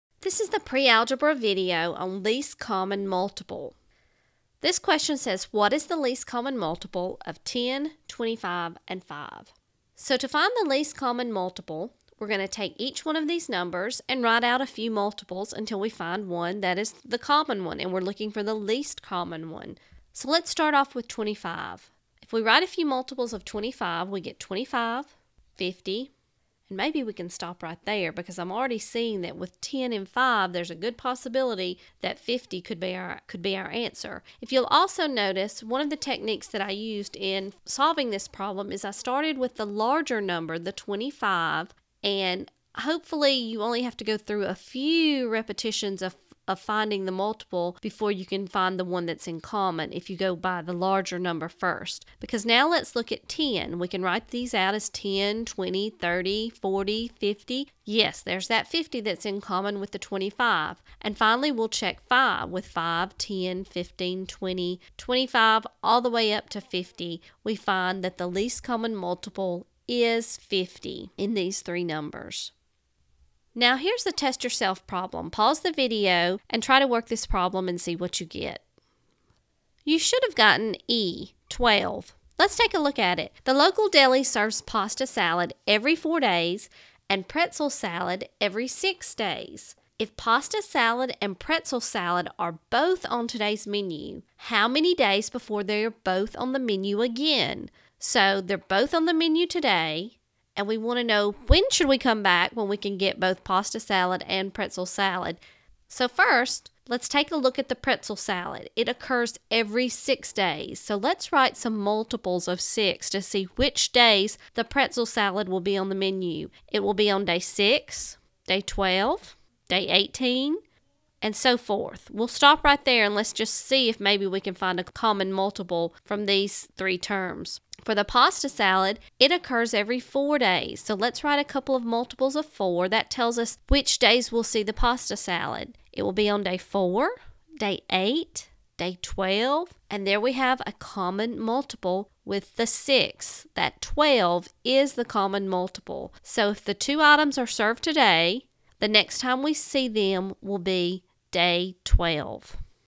Learn ACT Math - Finding the Least Common Multiple
Finding the least common multiple is a PreAlgebra skill for ACT practice problems.  The form of the questions for least common multiple (LCM) may appear as you being clearly asked to find the LCM or in the form of a word problem.  See both types in this video.